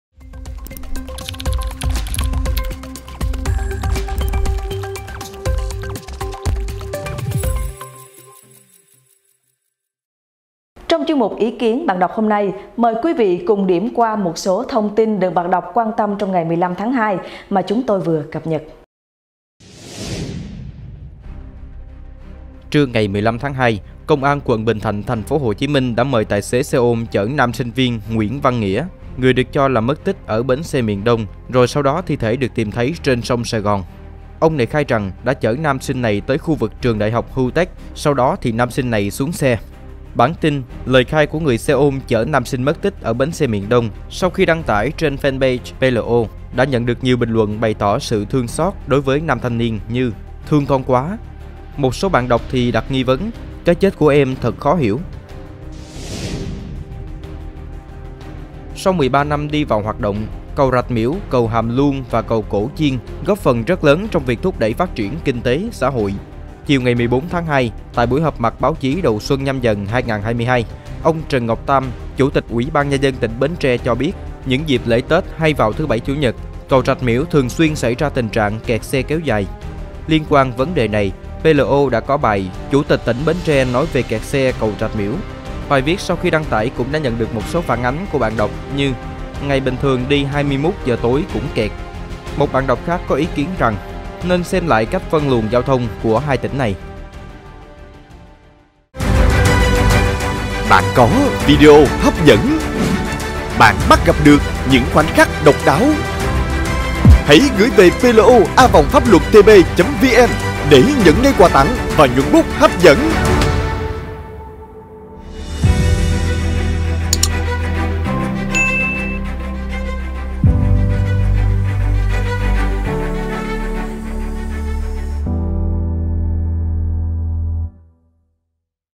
Ý kiến bạn đọc: Lời khai của người xe ôm chở nam sinh mất tích ở bến xe Miền Đông - PLO
Ý kiến bạn đọc 16-2: Lời khai của người xe ôm chở nam sinh mất tích ở bến xe Miền Đông; Chủ tịch tỉnh Bến Tre nói về kẹt xe cầu Rạch Miễu.